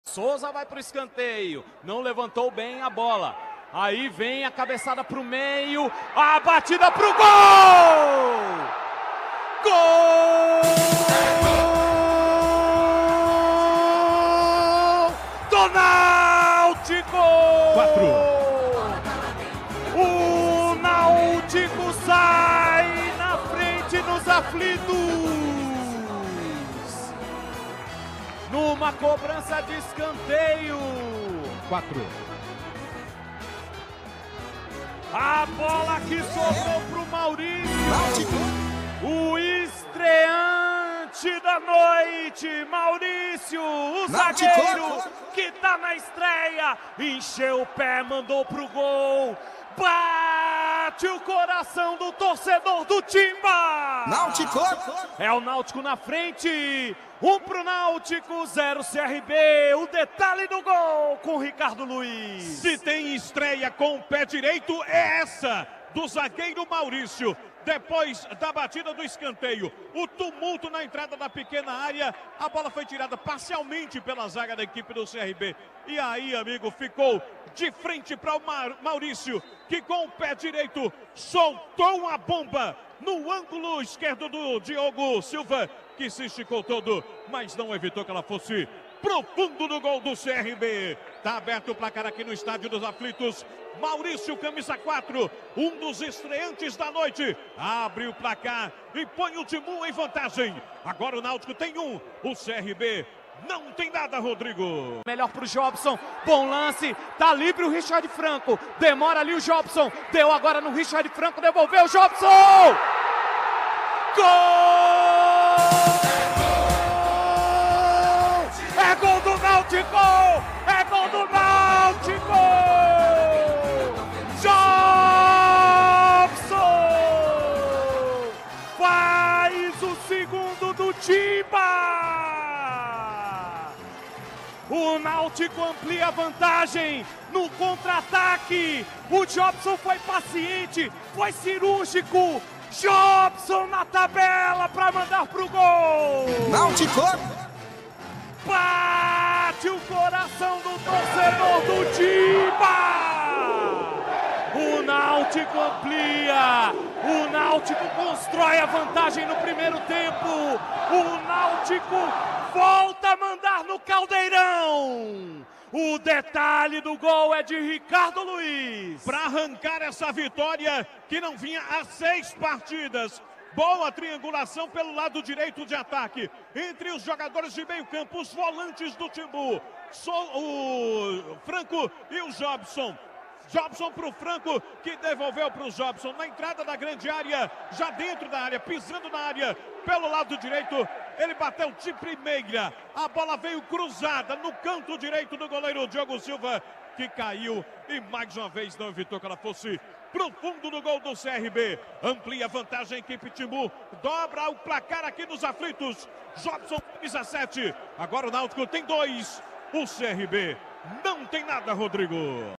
NÁUTICO 2 X 1 CRB/AL | NARRAÇÃO RODRIGO RAPOSO | CBN RECIFE
Ouça novamente a narração dos gols do Náutico diante do CRB pela 23º rodada da Serie B na voz de Rodrigo Raposo.

Nessa transmissão estiveram:
Narração: Rodrigo Raposo
Comentários: César Rosati
Reportagens: Ricardo Luís
Central da Bola: Vitor Araújo

Participe através do chat, whatsapp ou com a hashtag